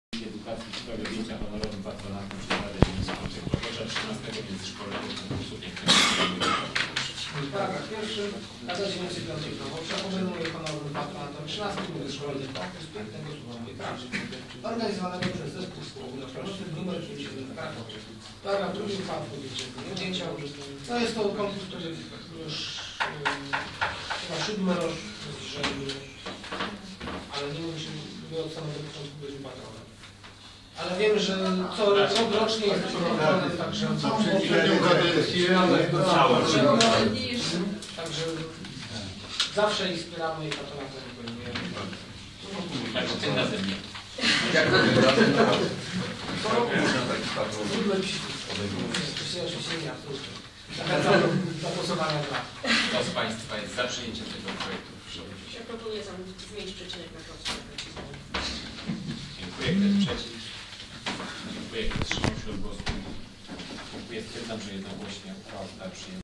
RD_V_22-10-2013_11 objęcie honorowym patronatem konkursu międzyszkolnego
Obrady XXXVII sesji Rady Dzielnicy V Krowodrza z dn. 22 października 2013 r. Uchwała w sprawie objęcia Honorowym Patronatem przez Radę Dzielnicy V Krowodrza XIII Międzyszkolnego Konkursu Pięknego Słowa „Mój Kraków".